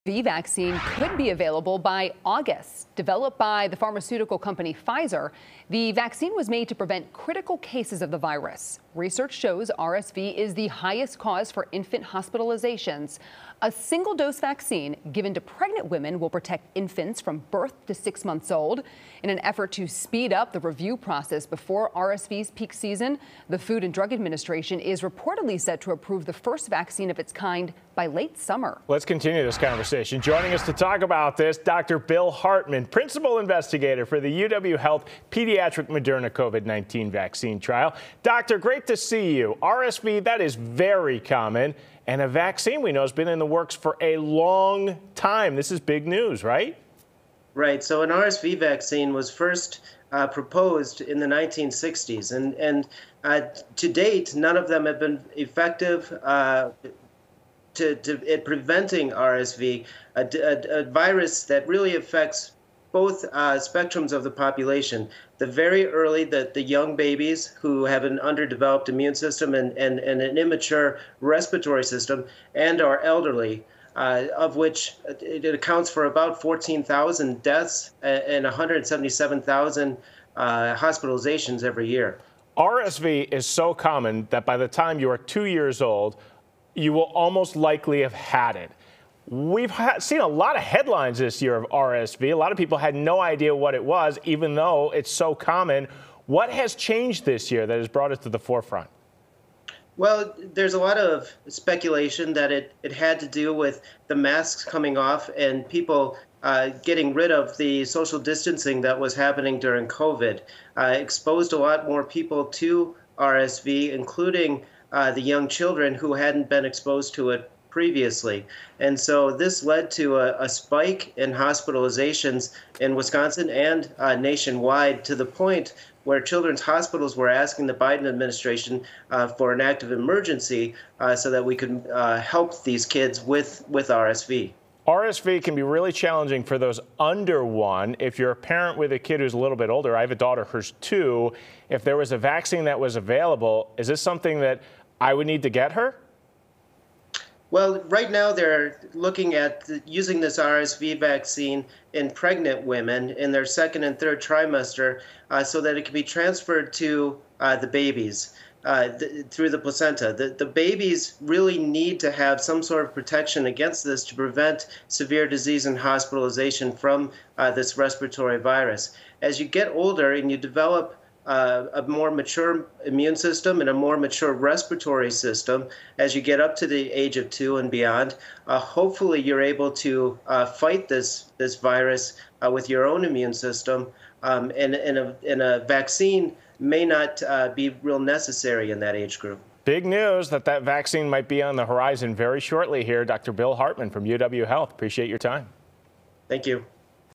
Expert weighs in on the making of the RSV vaccine | FOX6 News Milwaukee
Expert talks to FOX6 about the RSV vaccine and how it could change things.